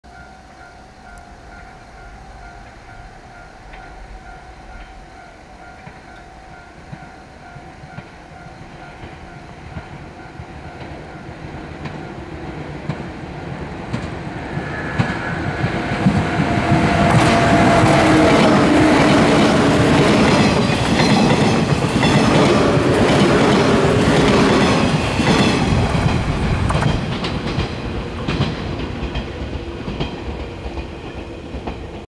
【4K】JR伯備線 普通列車115系電車 ｵｶD-21編成+ｵｶA-01編成
伯備線を走る115系電車です
単色化によって黄色くなっています
豪渓～総社間にて撮影